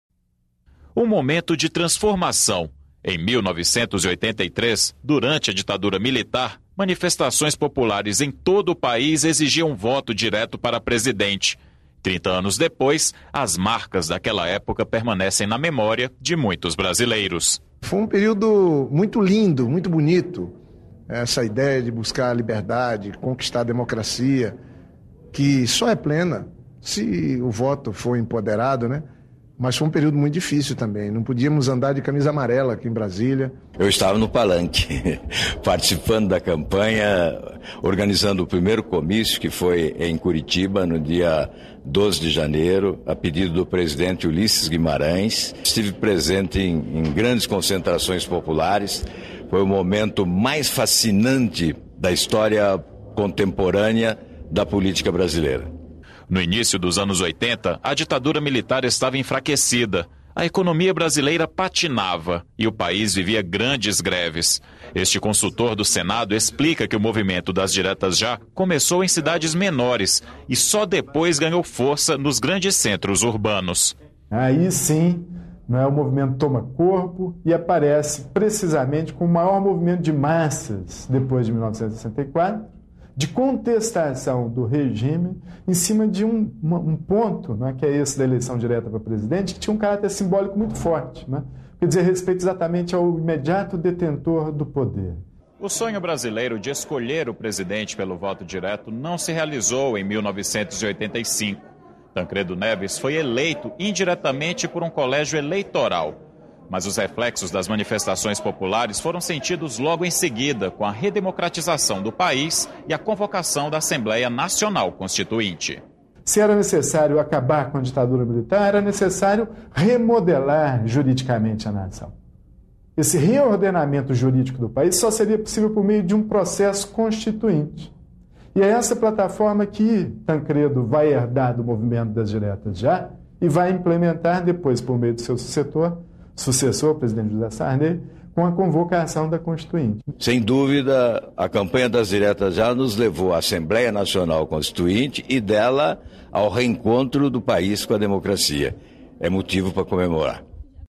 Senadores relembram campanha das Diretas
Trinta anos depois, as lembranças da campanha das diretas permanecem na memória de muitos senadores. "Foi um período muito difícil", lembra o senador Jorge Viana ao se referir ao uso da camisa amarela, cor símbolo da campanha. Para o senador Alvaro Dias, foi o momento mais fascinante da história contemporânea da política brasileira. Veja a reportagem de Hermes Coêlho, da TV Senado.